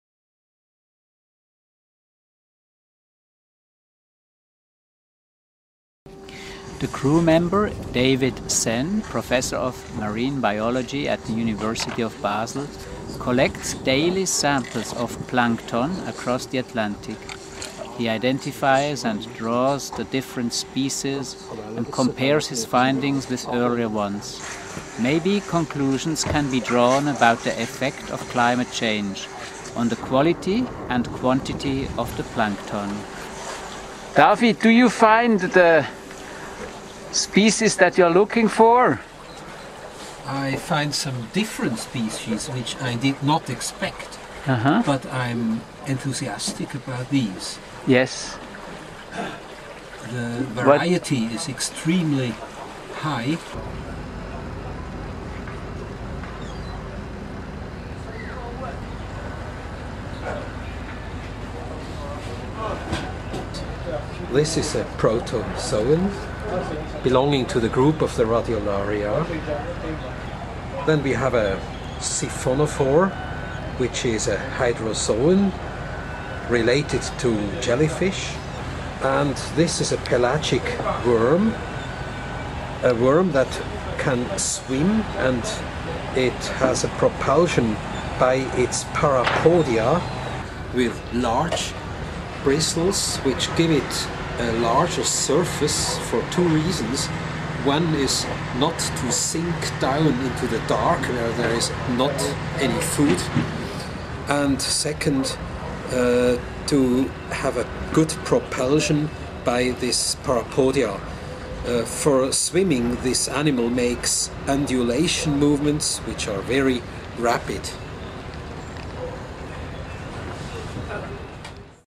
SUN21: PORTRAIT OF DAVID SENN
An interview with Professor at University of Basel, Switzerland.